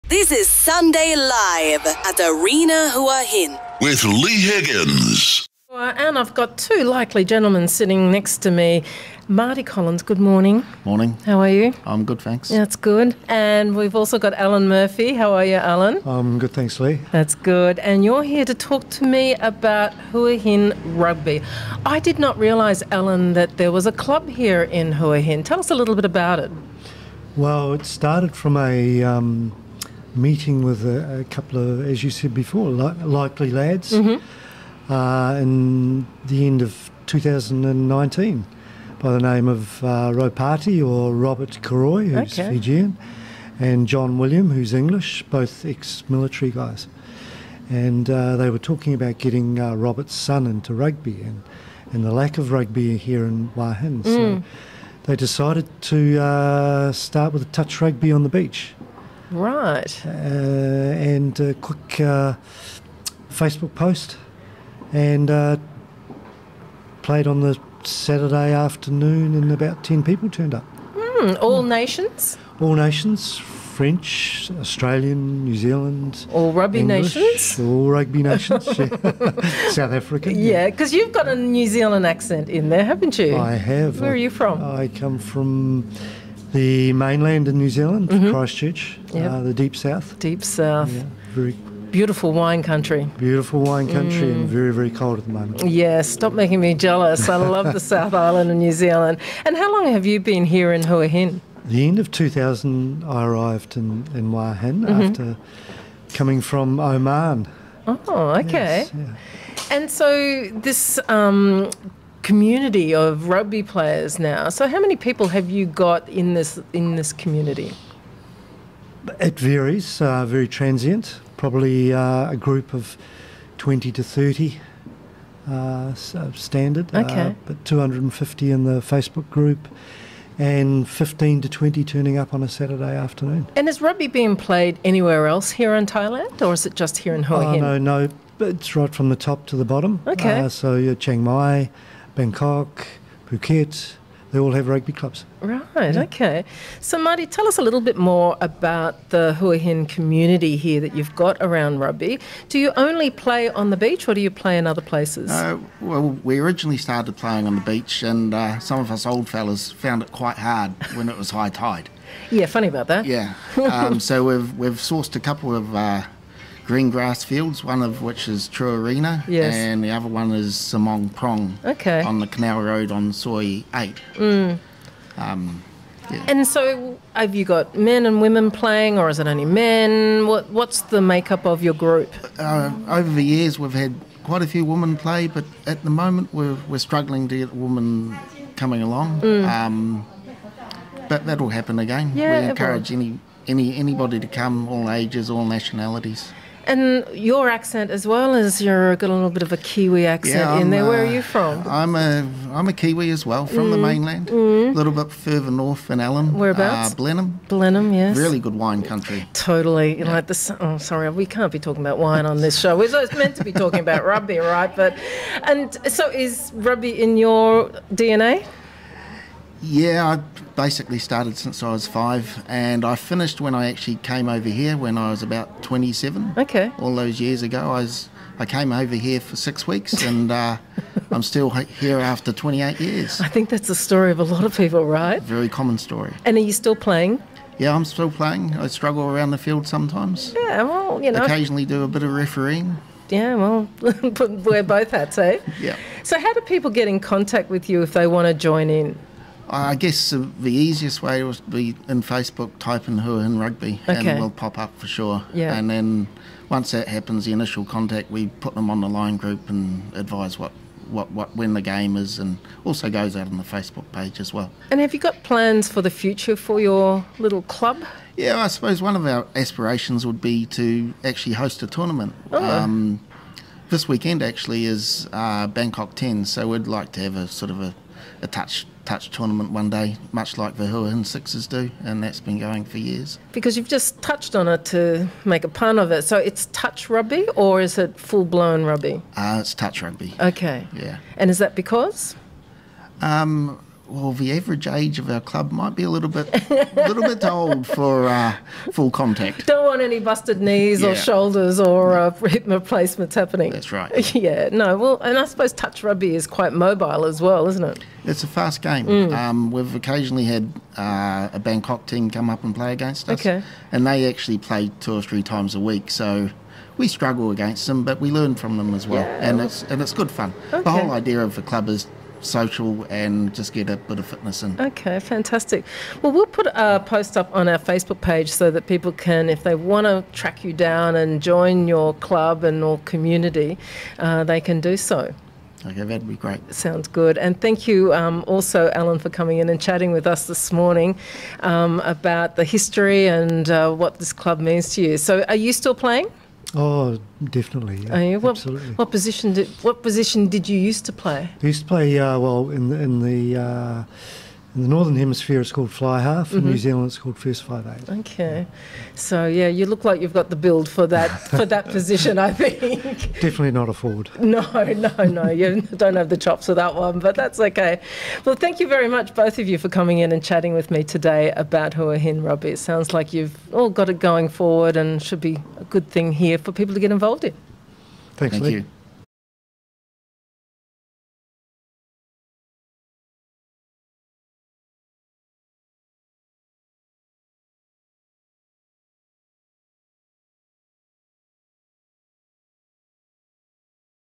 Surf Radio: Leigh and Huahin Rugby
Surf Radio Hua hin talking with fascinating people.
Leigh finds out about a local rugby club run by Marty Collins and Allen Murphy.